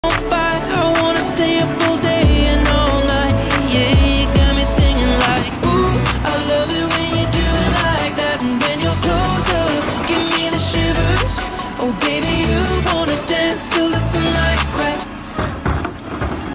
Brand New Garbage Truck-Mack LR-Republic Services 4038
one out of 11 new LR labrie exterts the Mt. Laurel got/getting.

Filmed 1/11/22